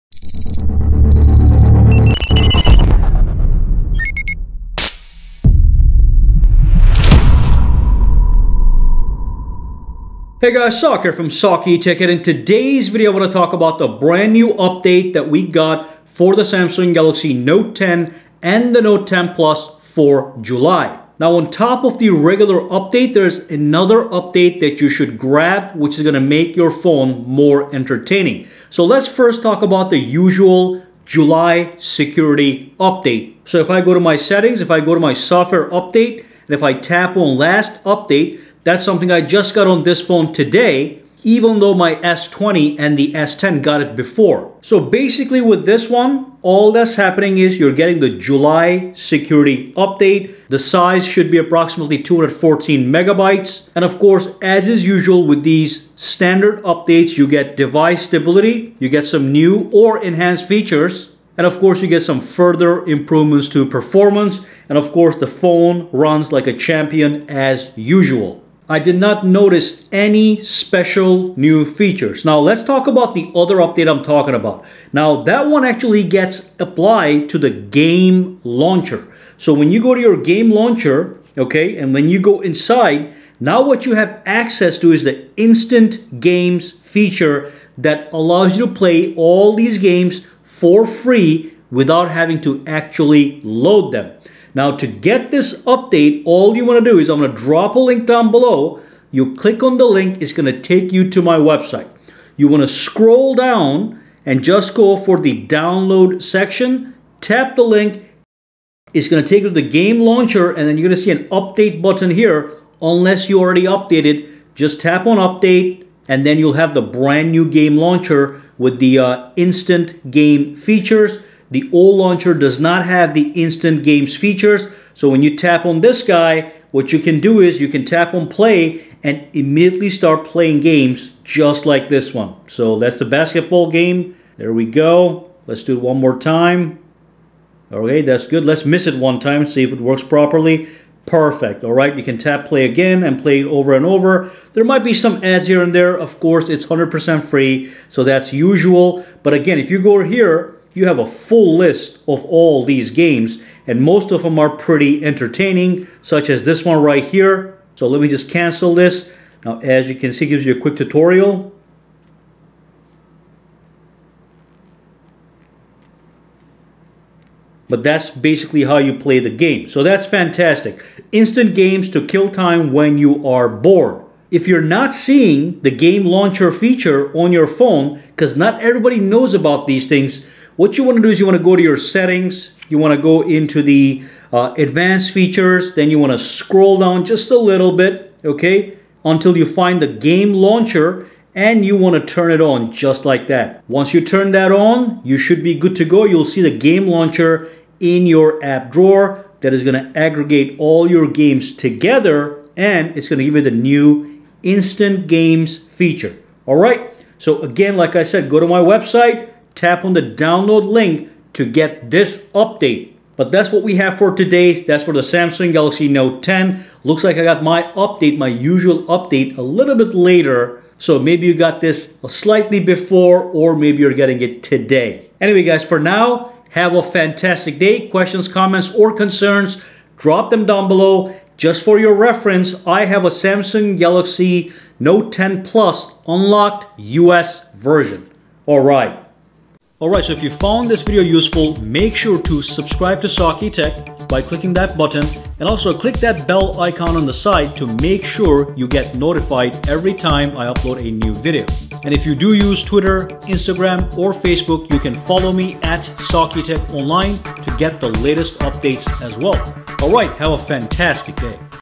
NEW Software Update for Samsung Galaxy Note 10 - What's New? (New Feature)
Samsung Galaxy Note 10 and 10 Plus get a brand new software update for July. Let's dive in and see what kind of new improvements and features we have received.

I own an unlocked Galaxy Note 10 Plus that was purchased directly from samsung in US region.

Companies can send product to the following address:

Attn: SAKITECH
135 S Springfield Rd. Unit #681